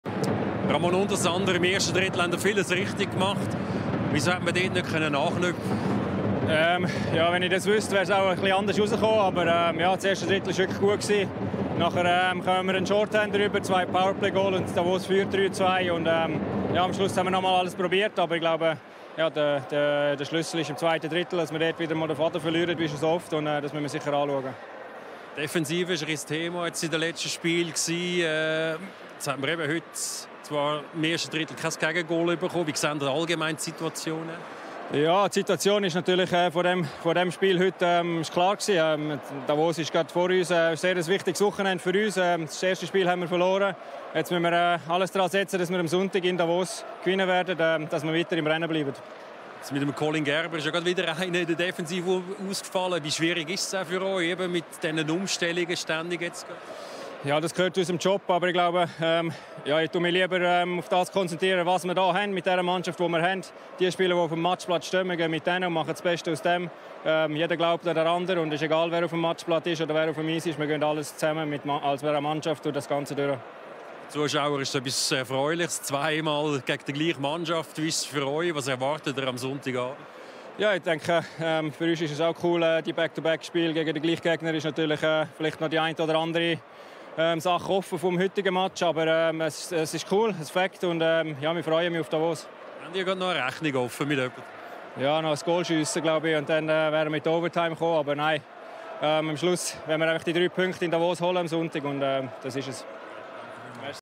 Ramon Untersander: «Wir haben wie so oft den Faden verloren.»
Ramon Untersander spricht nach der 2:4-Niederlage gegen Davos Klartext.